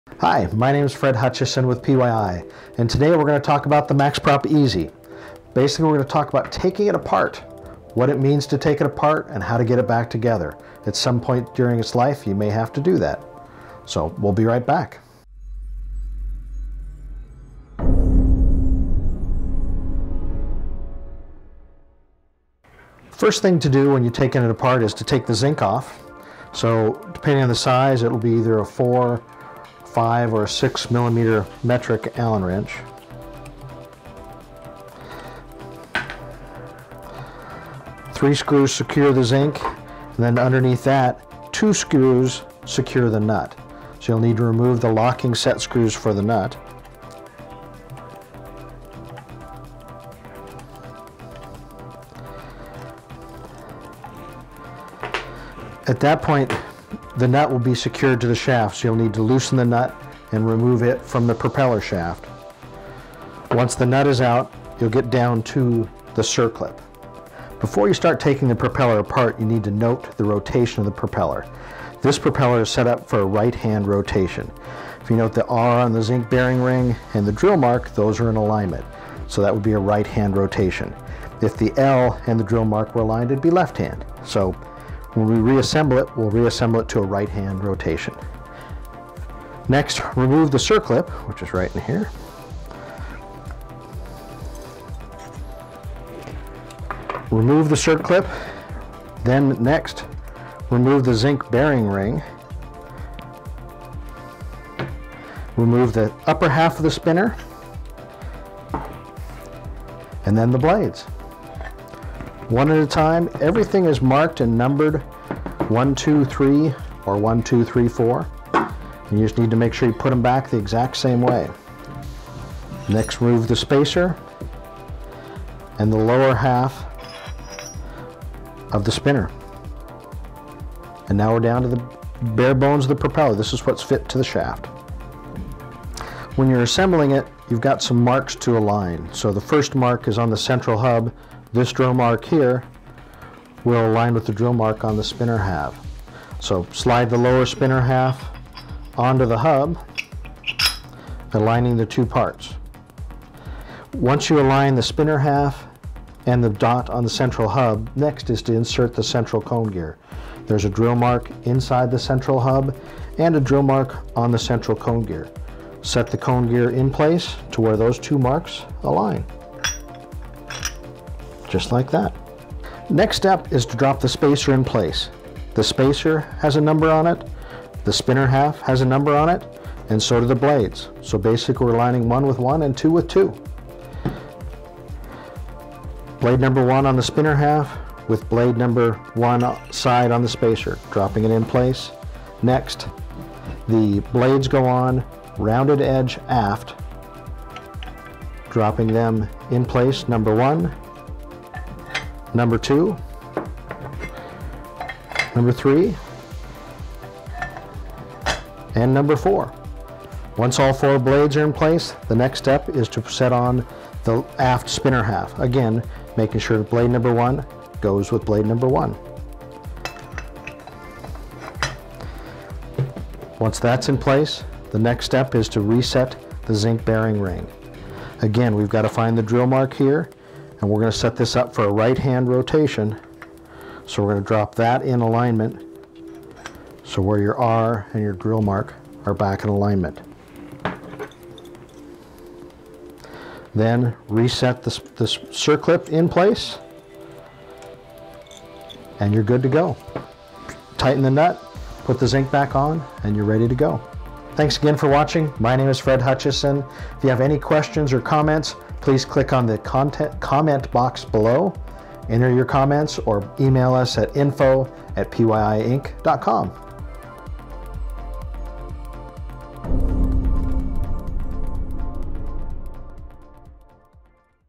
Max Prop Easy Disassembly
Want to know how to disassemble and reassemble a Max-Prop Easy automatic feathering propeller? In this video we show you step by step on how to totally disassemble your Max-Prop Easy and then put it all back together.